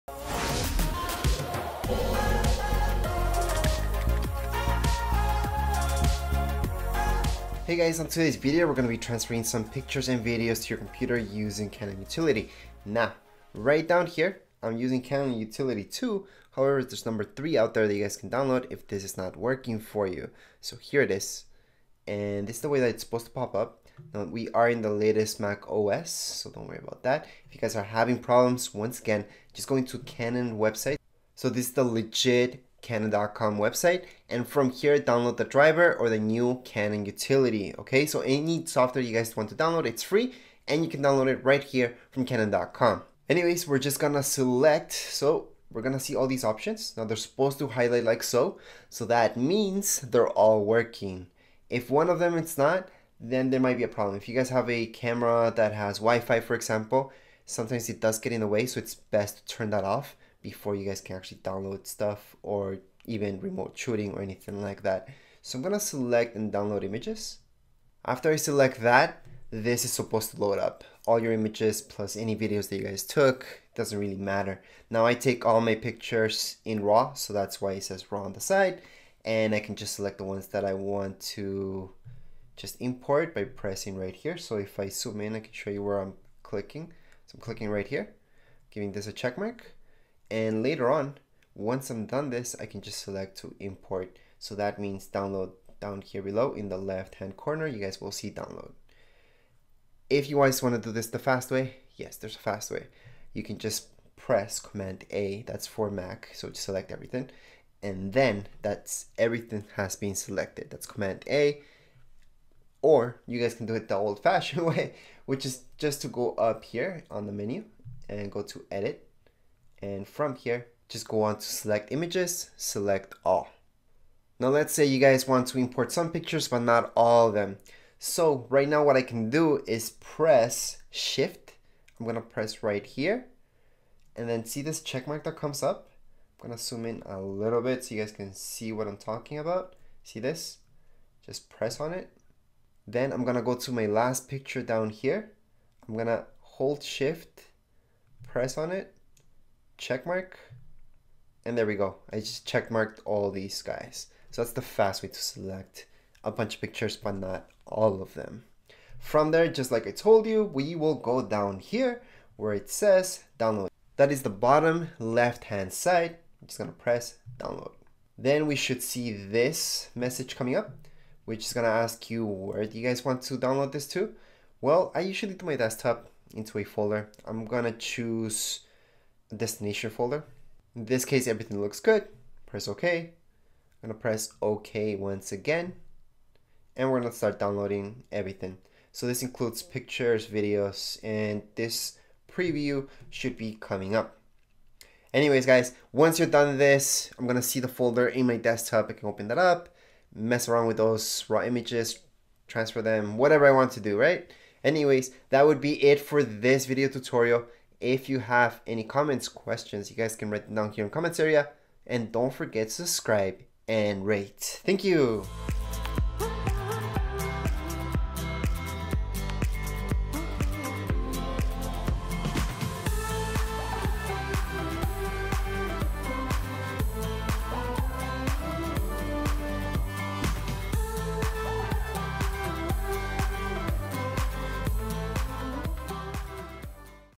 How to use Canon EOS Utility to Import pictures & videos on Mac | 2019
Use Canon software to transfer photos and videos to MacBook, iMac, Mac mini, Mac Pro